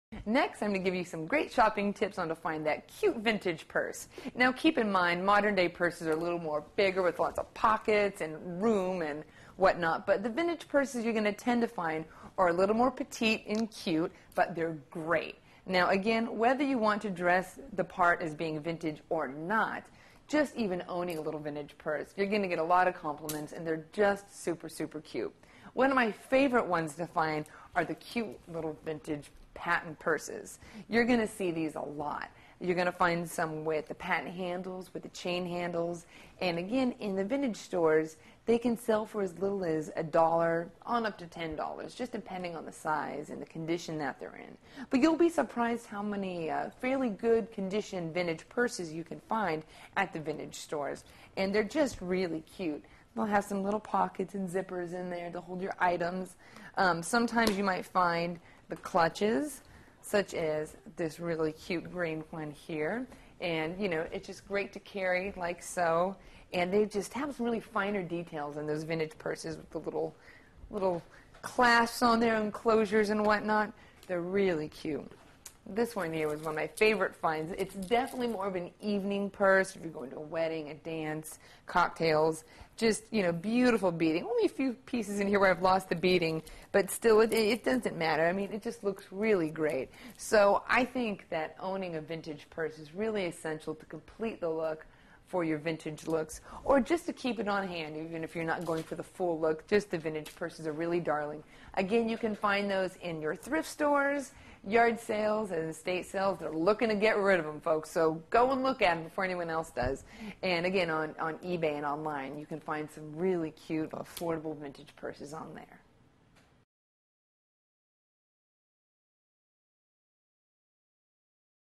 Creating a Vintage Look : Tips for Finding Vintage Purses
Learn about vintage purses to your outfit in this free video clip about retro clothing.

Expert: Amber Kloss
Bio: Amber Kloss has been collecting and obsessing over vintage clothing since her Great Grandmother gave her dress-up clothes. Amber now sells and trades her pieces. She also uses them for modeling.